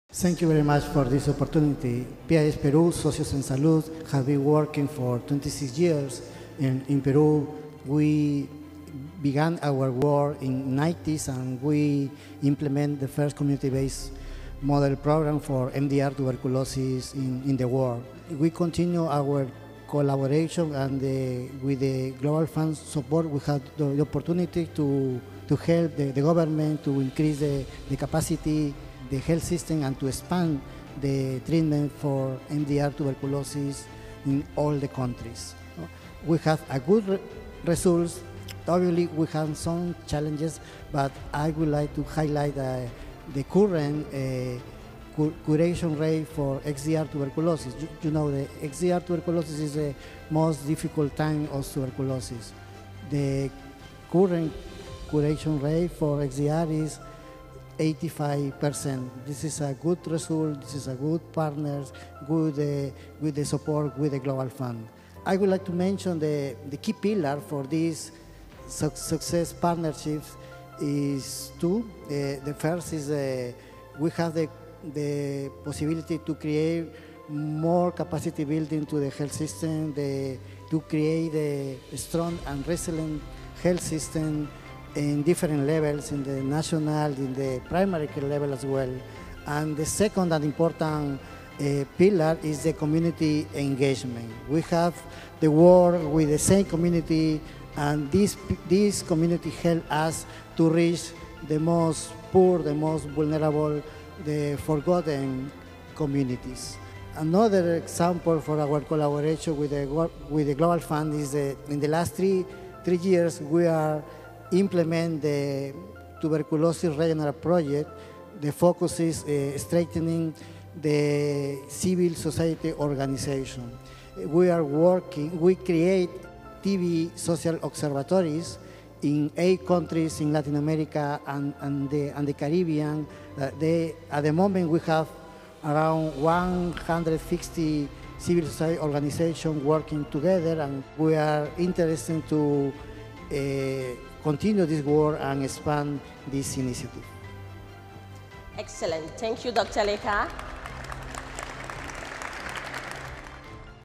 PHI PERÚ, SOCIOS EN SALUD ESTUVO EN LA 7MA CONFERENCIA DE REABASTECIMIENTO DEL FONDO MUNDIAL
El Dr. Leonid Lecca, director de Socios En Salud en Perú, fue invitado a este evento  para hablar sobre el alcance de las estrategias que ponen un alto a la tuberculosis,   gracias al financiamiento del Fondo Mundial en la 7ma Conferencia del Reabastecimiento del Fondo Mundial -2022.

Gracias a los aportes del Fondo Mundial a @partnersinhealth, @sociosensalud en Perú, podemos seguir luchando contra la tuberculosis e implementar estrategias que ayuden a los sistemas de salud con la detección oportuna de TB en más de 8 países en Latinoamérica, y así salvar miles de vidas en las comunidades más vulnerables.

Lee más sobre nuestro apoyo a las comunidades más vulnerables a través de nuestro Programa de Tuberculosis:
🧡Nuevo programa para transformar la respuesta contra la tuberculosis en Latinoamérica y el Caribe